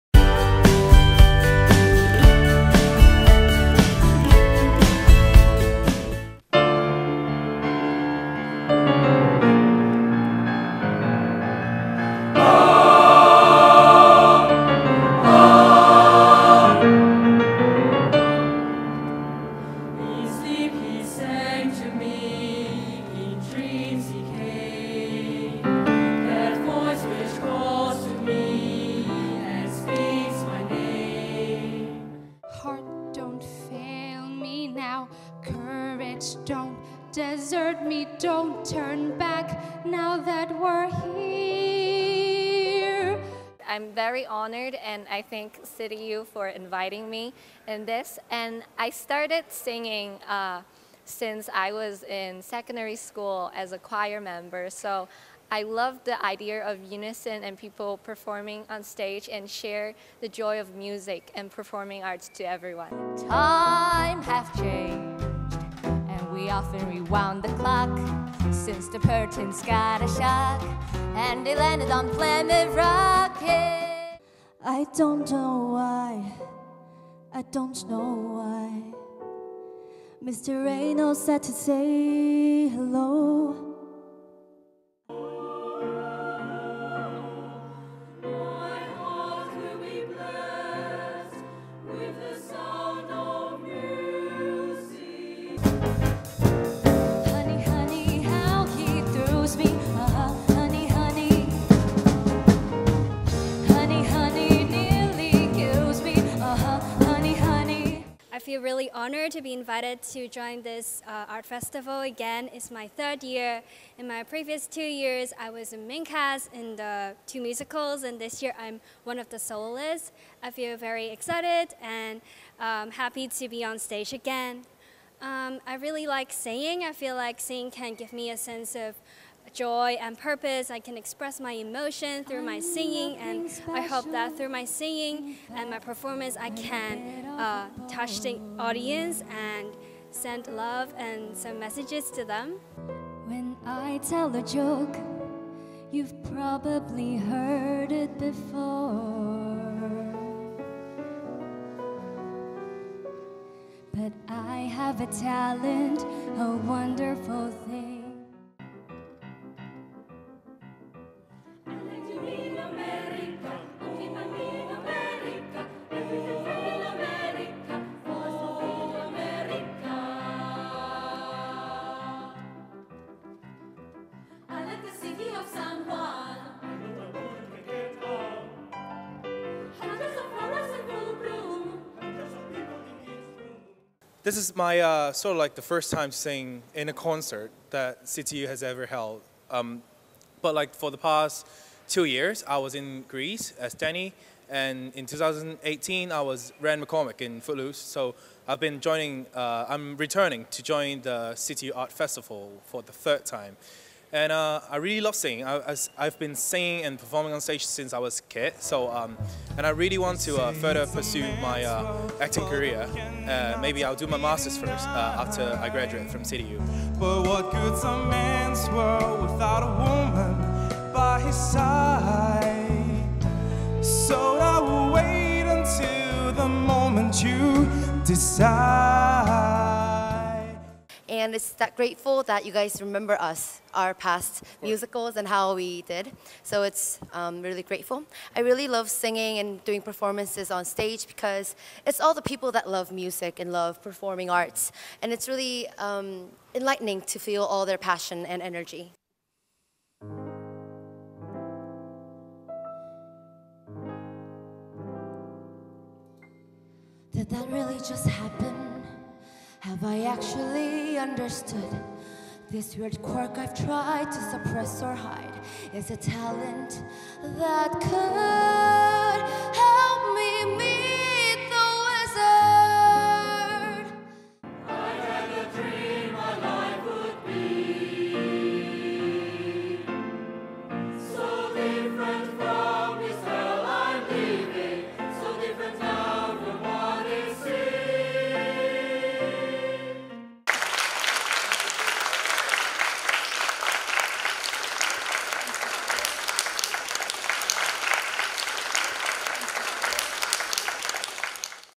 CityU Arts Festival 2019: Choir Concert
Date: 21 October 2019
Time: 19:30
Venue: Wong Cheung Lo Hui Yuet Hall, 6/F, LAU, CItyU